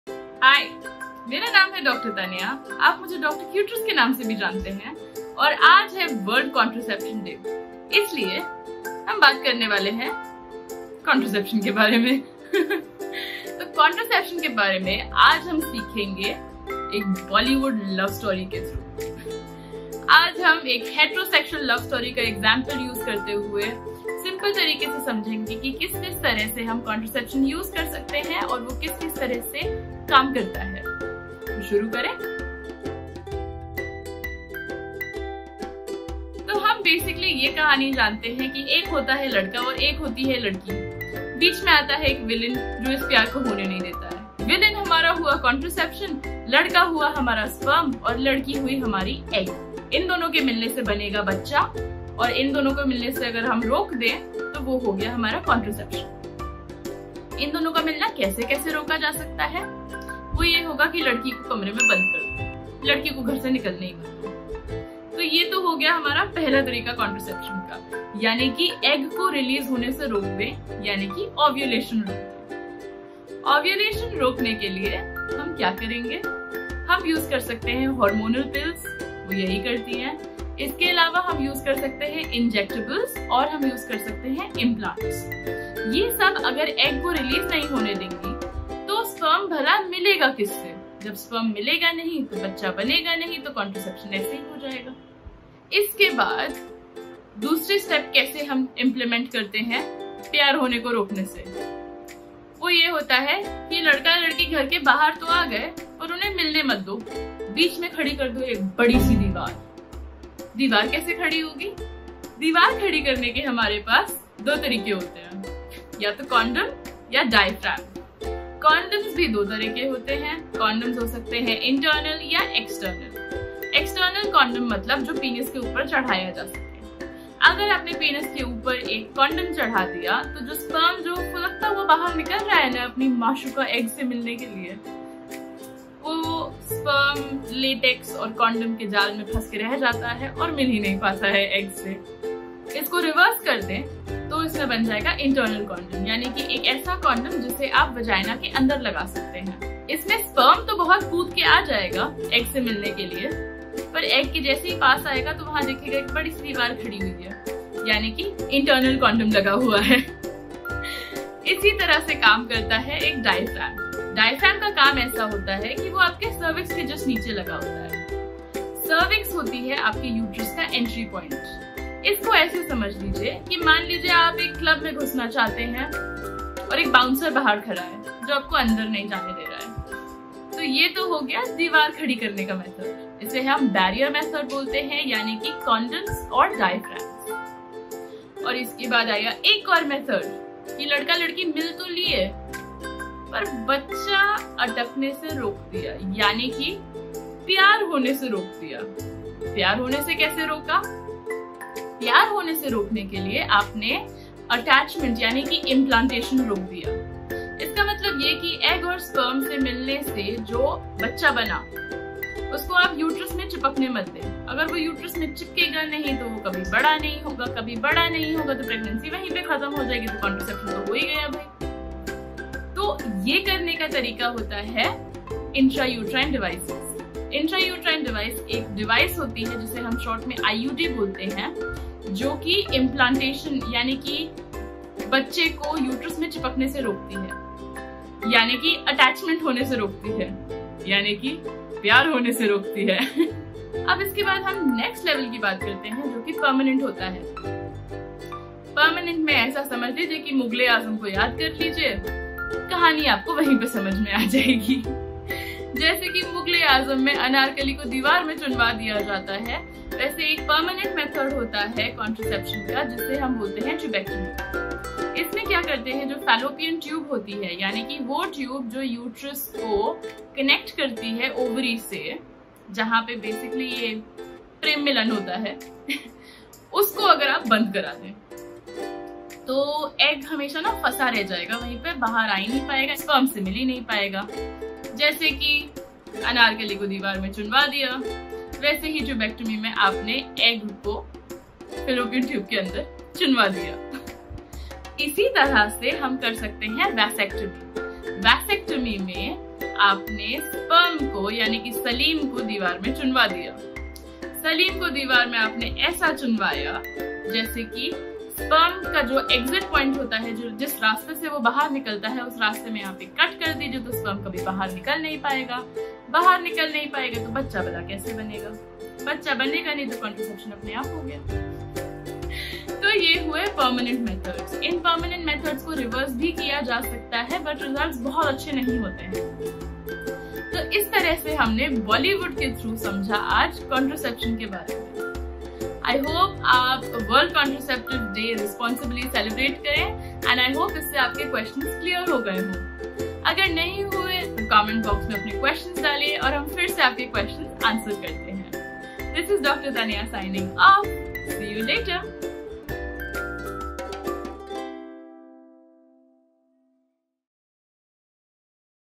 Contraception (Hindi) - Dr. Cuterus Explains
Contraception - nirodh, condoms, contraceptive (birth control) pills, vasectomy, tubectomy! Itne saare type ke contraception. But ye contraception method kaise work karte hain? I joined hands with @youthkiawaaz to talk all about contraception!

Putting the FUN in FUNdamental sex education, join me as I talk about how to prevent a pregnancy, sperm, eggs, condoms, vasectomy, tubectomy, tubal ligations, uteruses, and everything contraception!

Also, send them a pack of flavoured condoms for great oral sex! Byeeeeeee